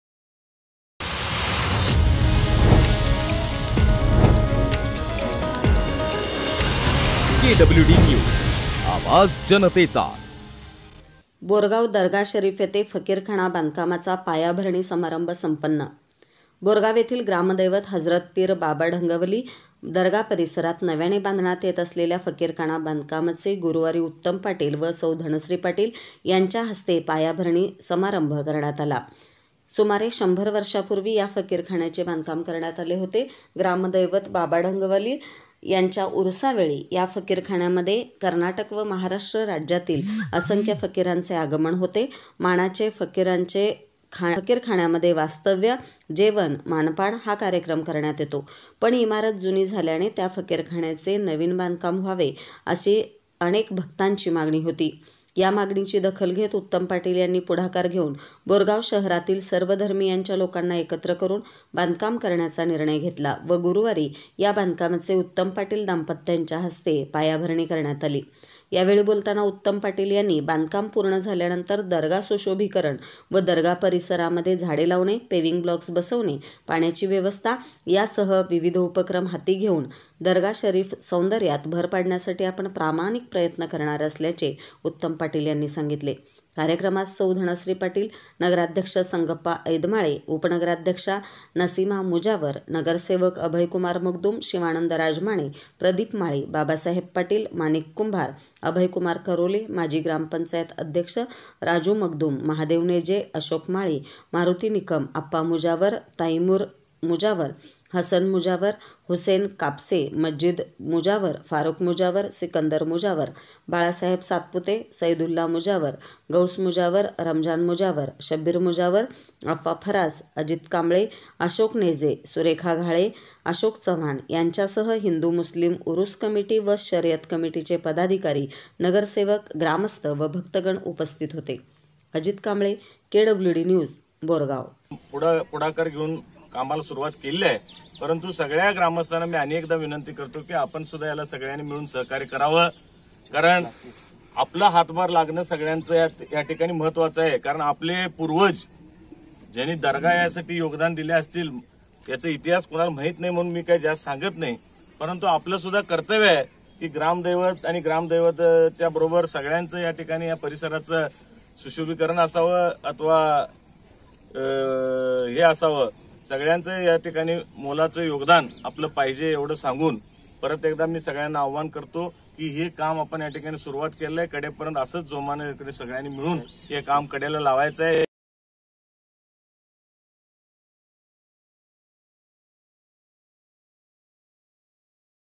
बोरगाव दर्गाशरीफ येथे फकीरखाना बांधकामाचा पायाभरणी समारंभ संपन्न
प्रतिनिधी : अजित कांबळे, बोरगांव